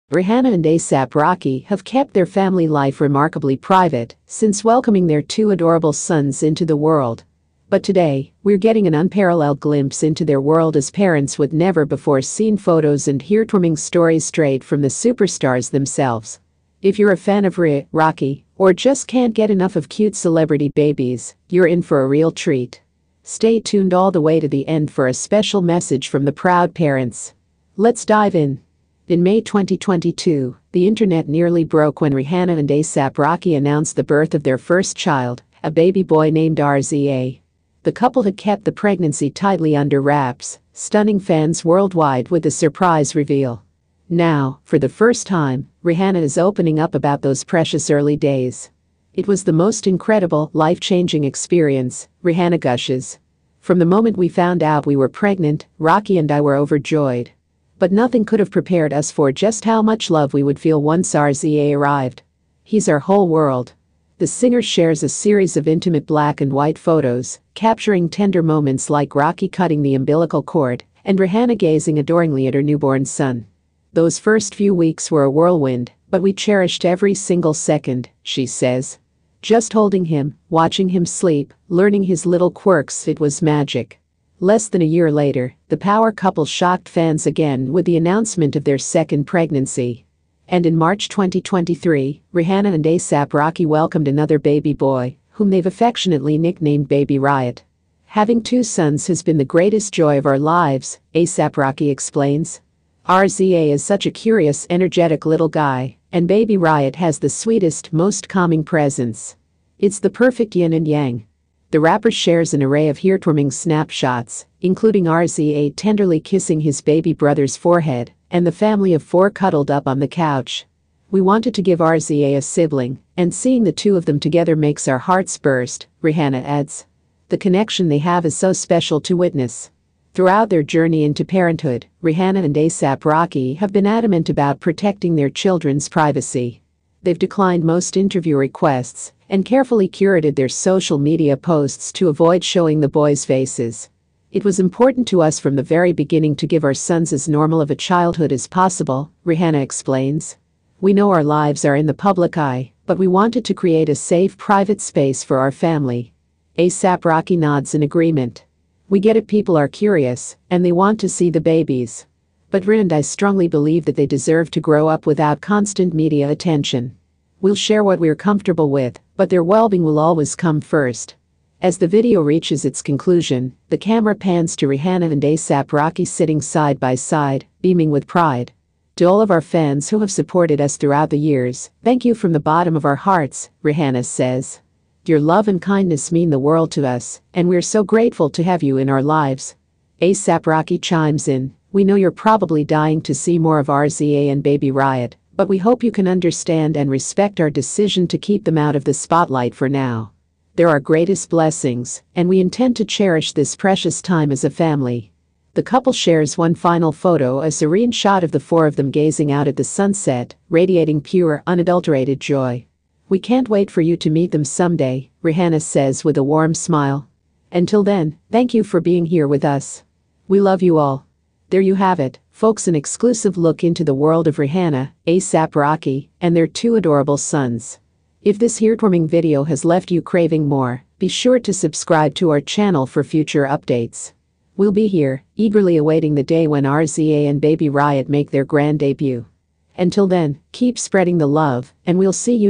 Rihanna & ASAP Rocky Shares Never Before Seen Snaps of Sons RZA & Baby Riot
today, we're getting an unparalleled glimpse into their world as parents - with never-before-seen photos and heartwarming stories straight from the superstars themselves. If you're a fan of Rih, Rocky, or just can't get enough of cute celebrity babies, you're in for a real treat. Let's dive in! In May 2022, the internet nearly broke when Rihanna and ASAP Rocky announced the birth of their first child, a baby boy named RZA. The couple had kept the pregnancy tightly under wraps, stunning fans worldwide with the surprise reveal. Now, for the first time, Rihanna is opening up about those precious early days. "It was the most incredible, life-changing experience," Rihanna gushes. "From the moment we found out we were pregnant, Rocky and I were overjoyed. But nothing could have prepared us for just how much love we would feel once RZA arrived. He's our whole world." The singer shares a series of intimate black-and-white photos, capturing tender moments like Rocky cutting the umbilical cord and Rihanna gazing adoringly at her newborn son.